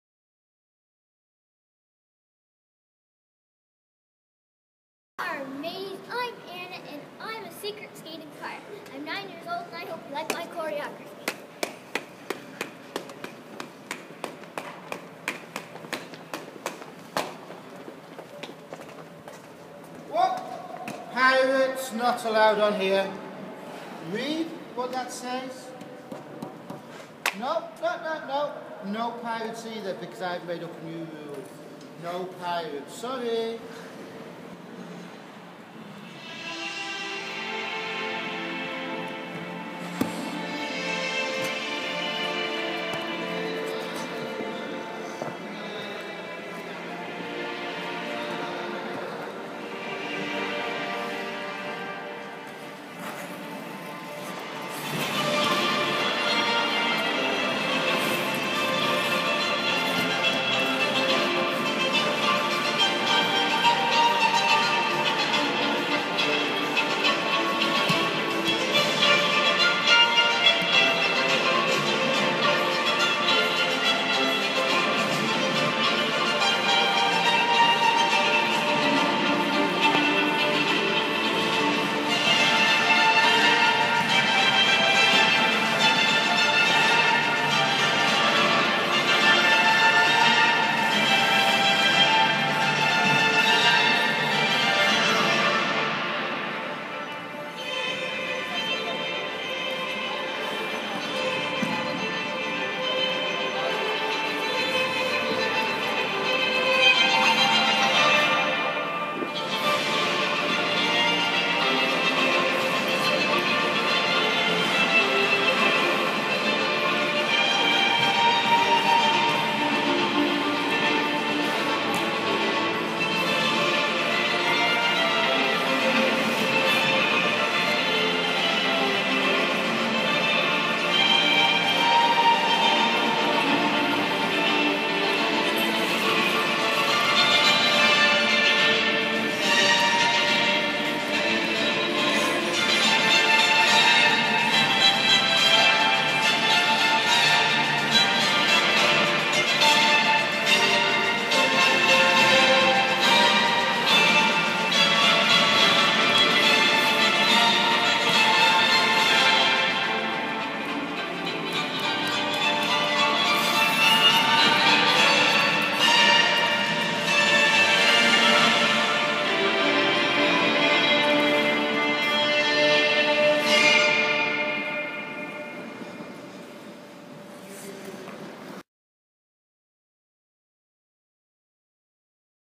Anna's Pirate Quest 2012.wmv
This is Anna Dale performing her quest choreography.